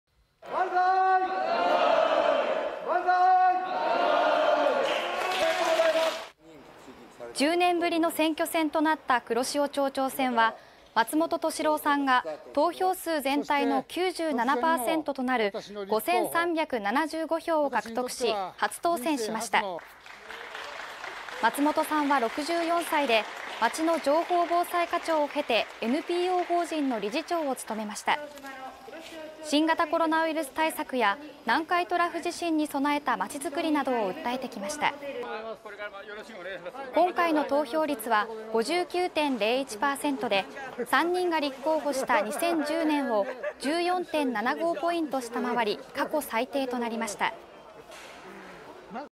黒潮町長選で松本氏が初当選 前町長辞任に伴い10年ぶりの選挙戦【高知】 (20/10/05 11:00)
前の町長の辞任に伴い10年ぶりの選挙戦となった黒潮町長選は4日、投開票が行われ、無所属で新人の松本敏郎さん(64)が投票数全体の97％となる5375票を獲得し、初めての当選を果たしました。
松本さんは町の情報防災課長を経てNPO法人の理事長を務めました。選挙では新型コロナウイルス対策や南海トラフ地震に備えた町づくりなどを訴えてきました。
今回の投票率は59．01％で、3人が立候補した2010年を14．75ポイント下回り過去最低となりました。